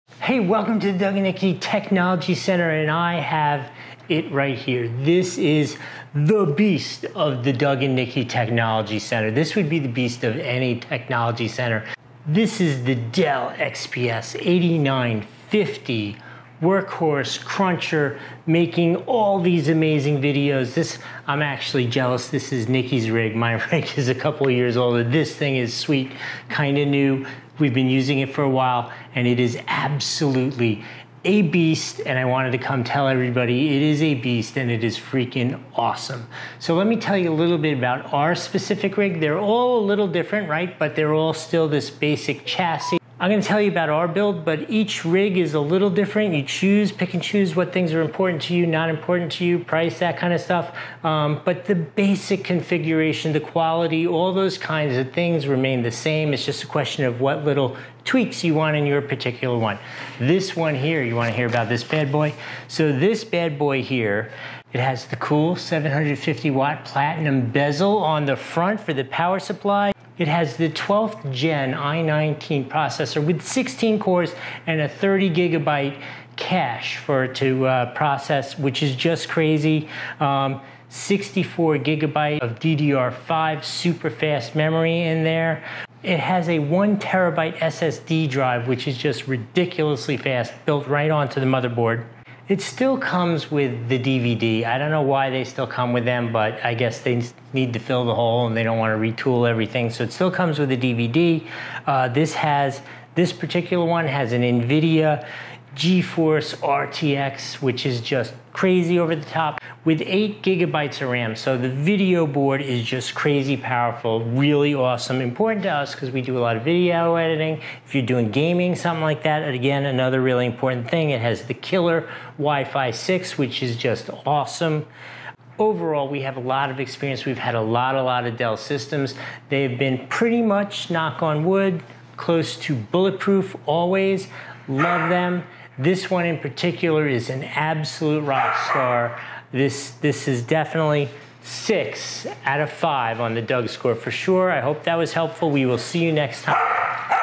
Reviewed: Dell XPS 8950 Desktop Computer
Dell XPS 8950 Desktop Computer - 12th Gen Intel Core i7-12700, 32GB DDR5 RAM, 512GB SSD + 1TB HDD, Intel UHD 770 Graphics, Killer Wi-Fi 6, Air Cooling, USB-C, Bluetooth, Windows 11 Pro - Black

PEAK PERFORMANCE: Create and multi-task with ease on this Dell XPS desktop computer, with the help of a 12th generation Intel Core i7-12700 processor
INCREDIBLE GRAPHICS: The Intel UHD Graphics 770 is designed for desktop computers and allows for faster speeds when editing photos or playing games
OPTIMIZED COOLING: Improved internal design and optional liquid cooling help you keep the pace you want, without overheating and keeping noise to a minimum
MINIMALIST ELEGANCE: Sleekly designed and crafted with premium materials, the XPS desktop computer complements you, your home, or your workspace